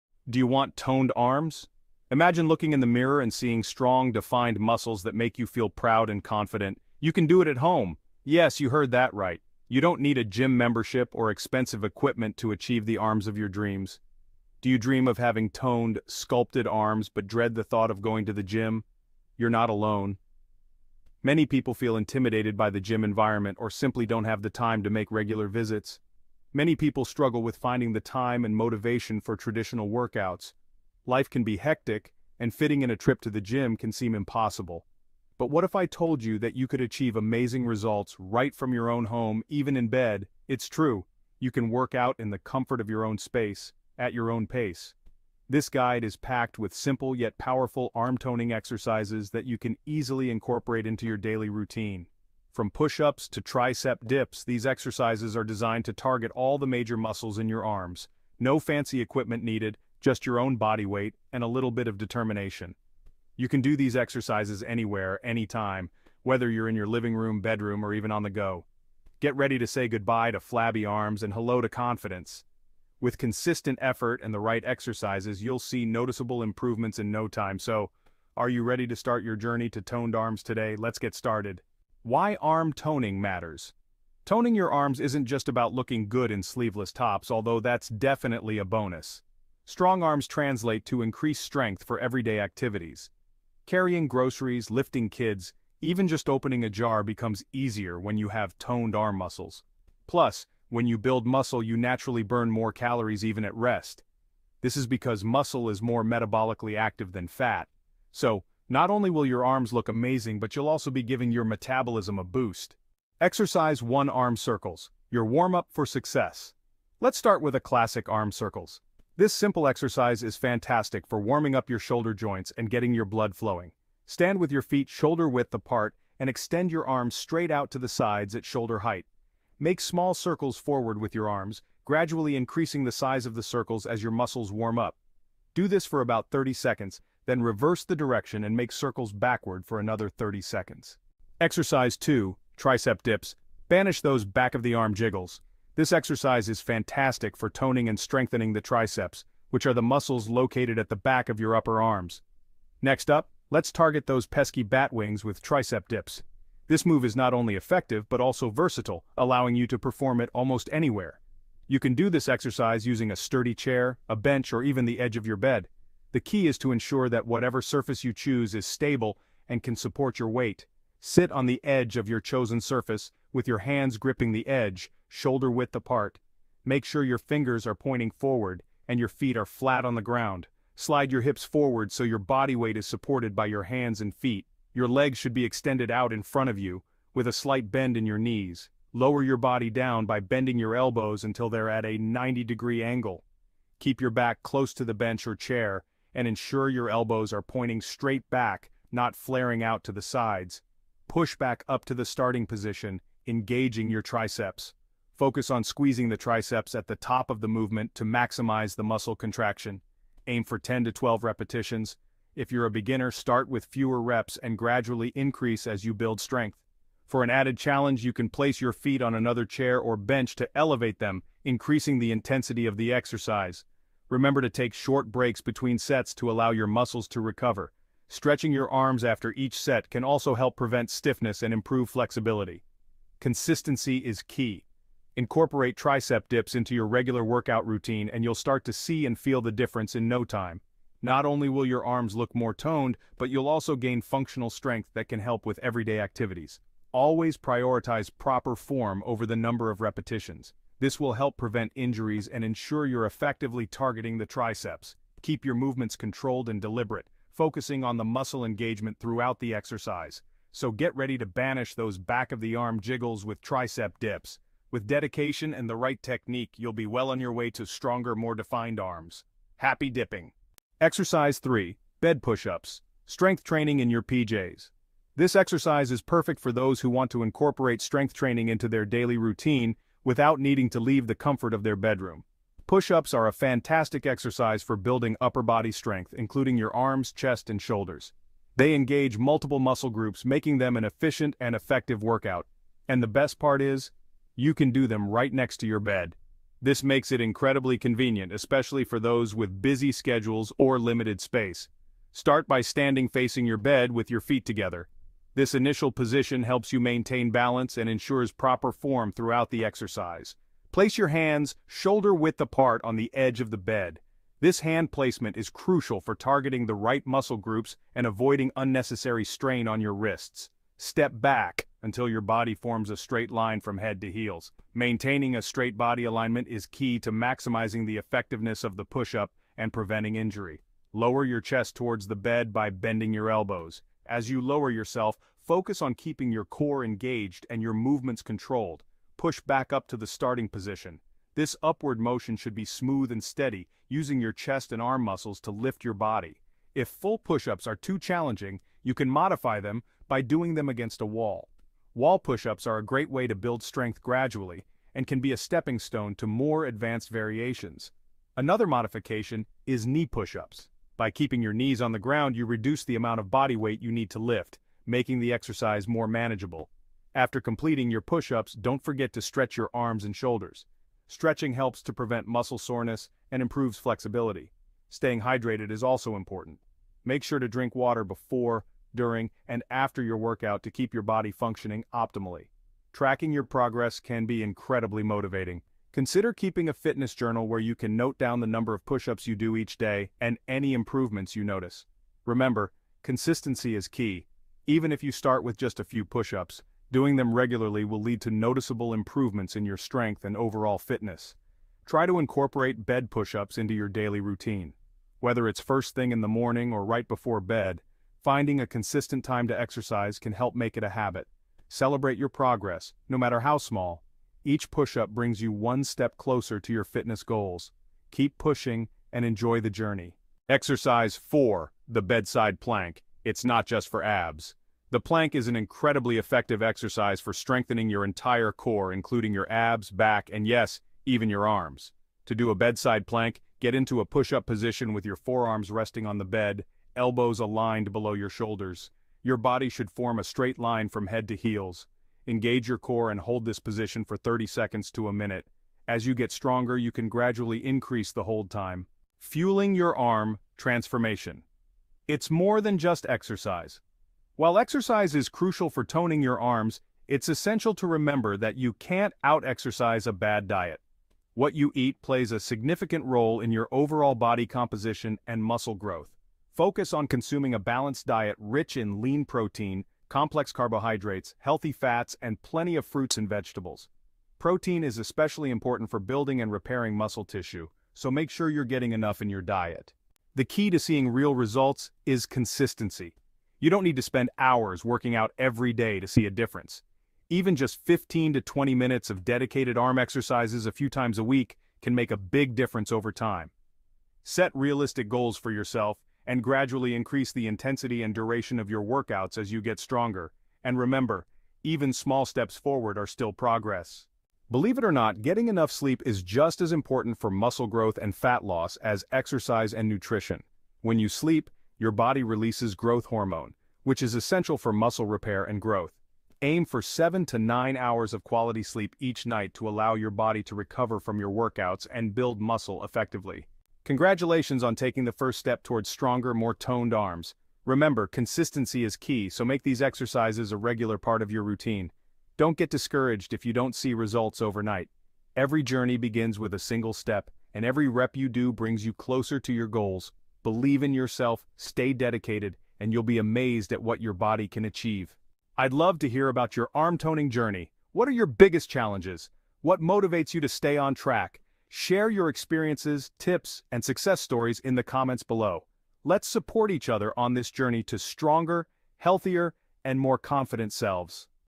Arm Toning Tips: How to Lose Weight in Your Arms
Looking to lose arm fat and tone your arms quickly? In this video, we share effective arm toning tips and workouts that you can easily do at home, even in bed! Whether you're wondering how to slim arms or seeking the best exercises to get toned arms in bed, we've got you covered. Learn how to lose flabby arms and discover the upper body workouts that help you achieve slim arms in as little as 2 weeks. These arm toning exercises require no weights and are perfect for anyone looking to lose weight and get toned arms fast. Start your journey to toned arms with these exercises to lose weight and see super fast results in just one week!

OUTLINE:
00:00:00 Do You Want Toned Arms?
00:01:21 Why Arm Toning Matters
00:01:52 Arm Circles - Your Warm-up for Success
00:02:21 Tricep Dips - Banish Those Back-of-the-Arm Jiggles!
00:04:30 Bed Push-Ups - Strength Training in Your PJs
00:07:00 The Bedside Plank - It's Not Just for Abs!
00:07:29 It's More Than Just Exercise
00:08:00 Make These Exercises a Habit
00:08:24 Your Secret Weapon for Muscle Recovery and Fat Loss
00:08:43 You've Got This! Embrace the Journey to Toned Arms
00:09:05 Share Your Success Story!